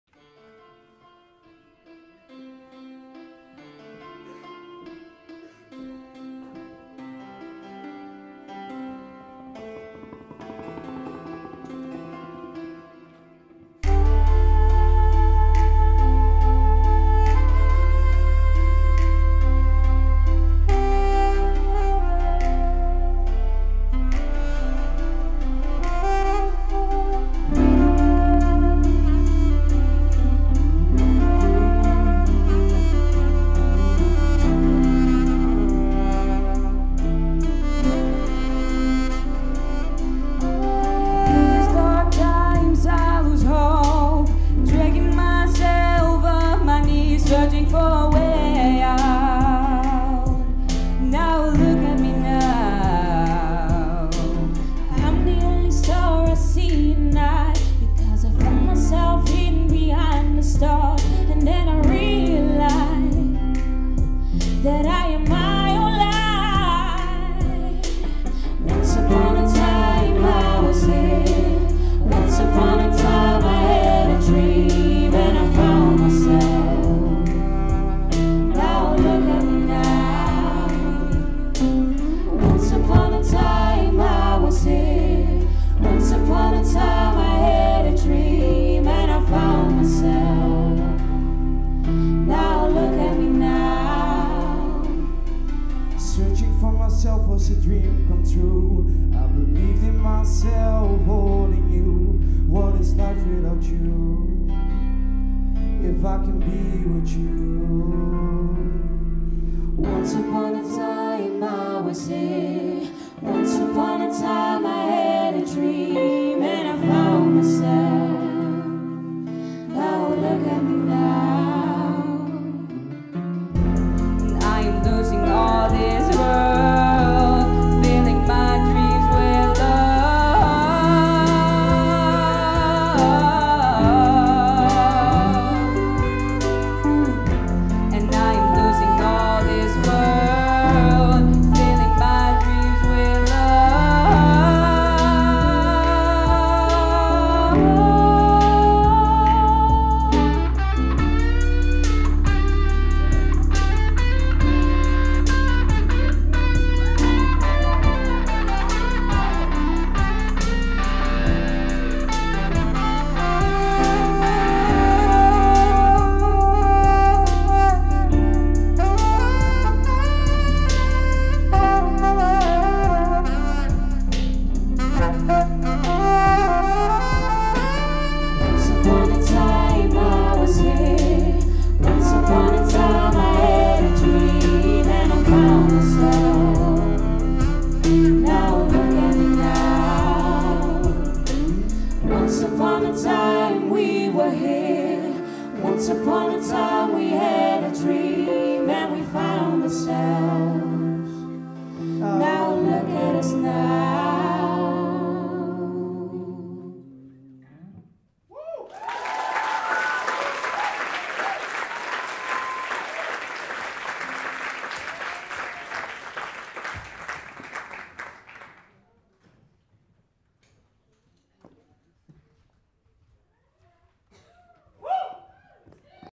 R&R Summerscool 2017: 007 Hello Inkt Project
ROOTS & ROUTES Summerscool 2017 Final Performance with the theme "Heroes", all music, dance and visuals created by the participants.